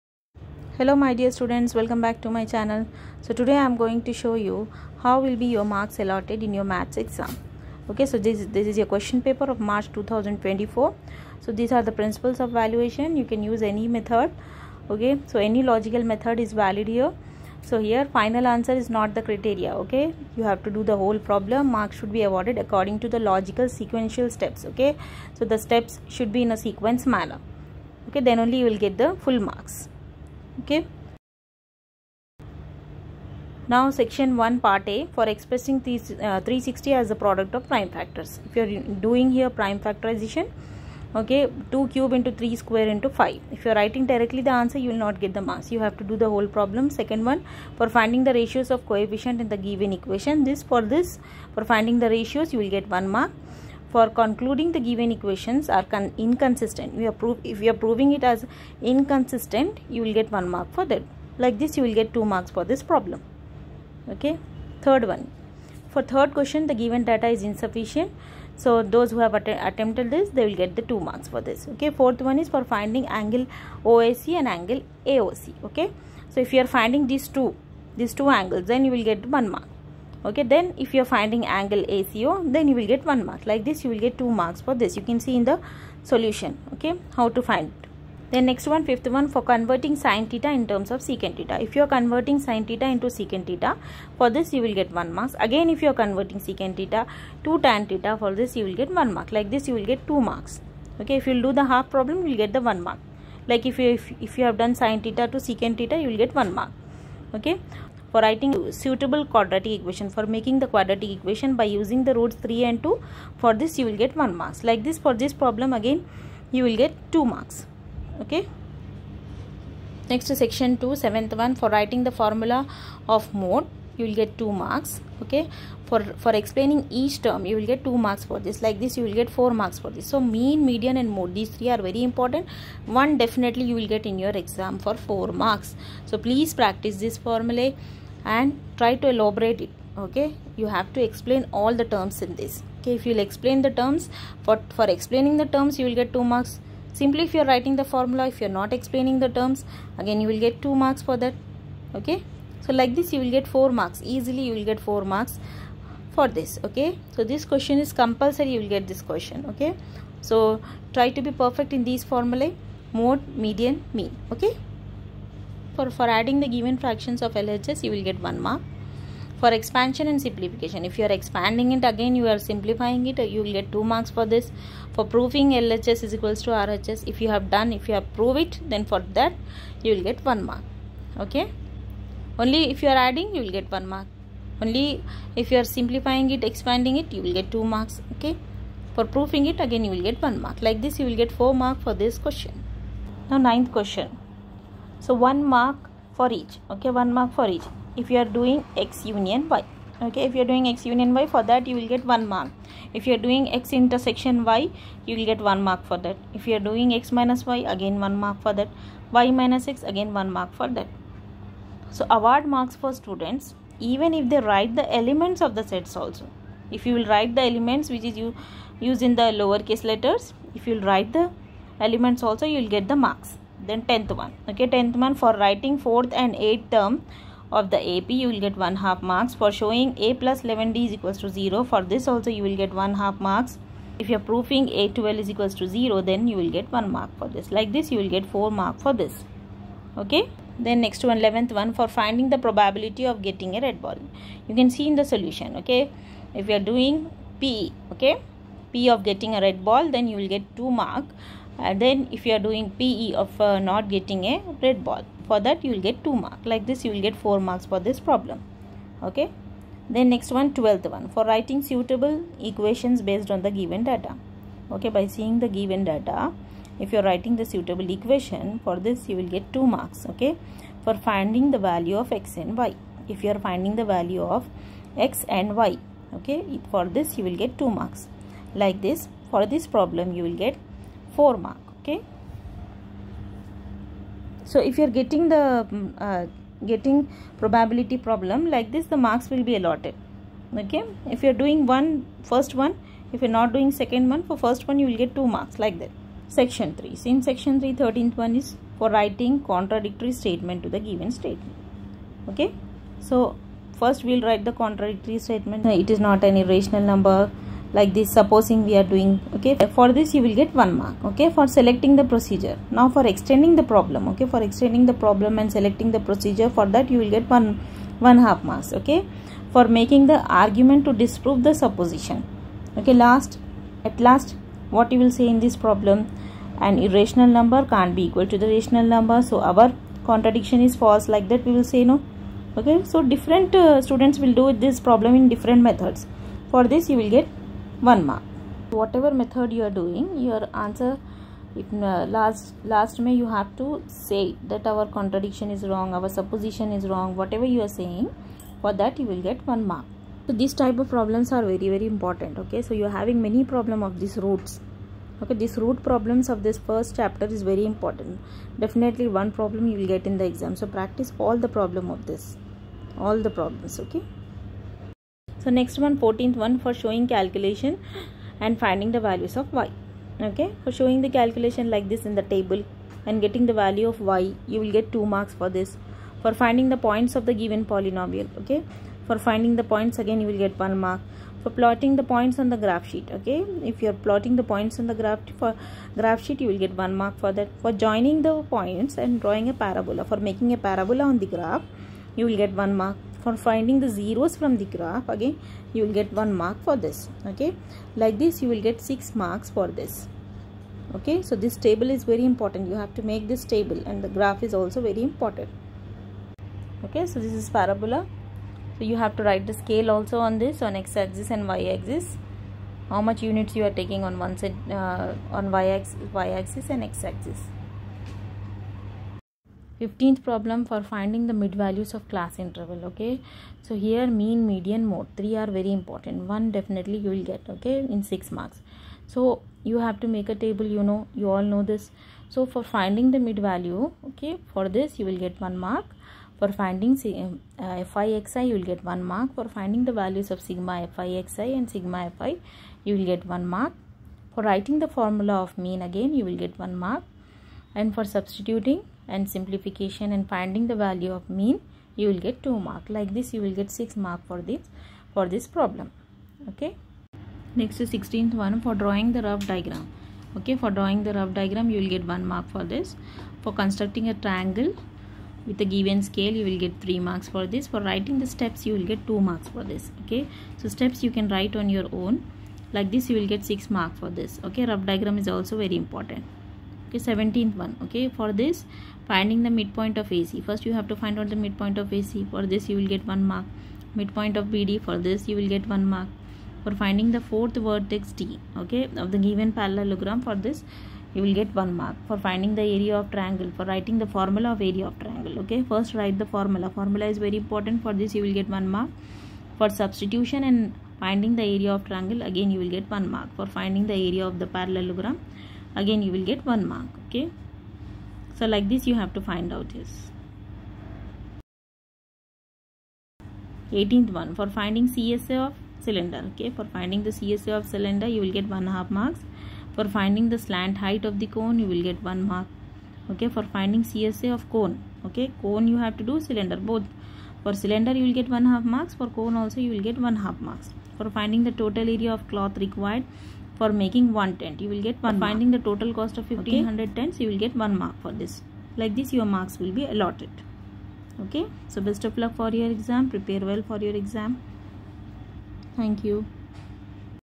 HOW MARKS ARE ALLOTED IN 10TH CLASS MATHEMATICS SSC PAPER | GoldenTutorial
you have to write the exam exactly how I explained in the video because this video shows how your marks are evaluated for your paper